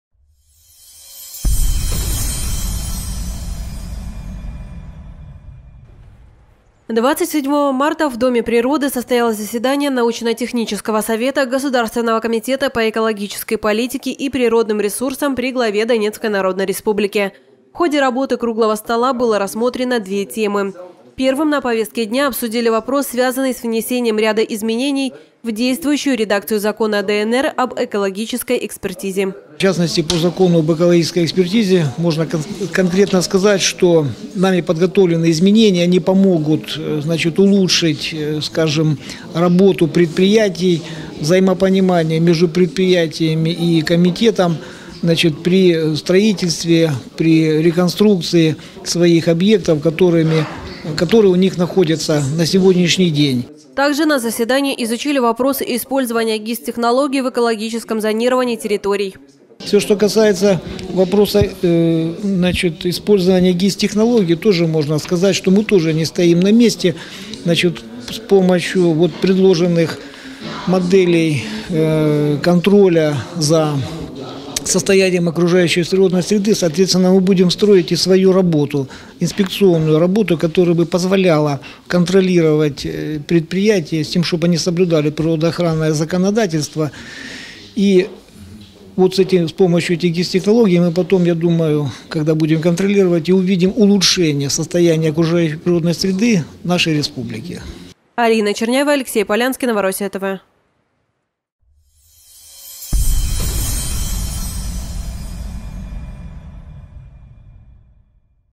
Заседание научно-технического совета Госкомэкополикити при Главе ДНР
27 марта в доме природы состоялось заседание научно-технического совета Государственного комитета по экологической политике и природным ресурсам при Главе Донецкой Народной Республики.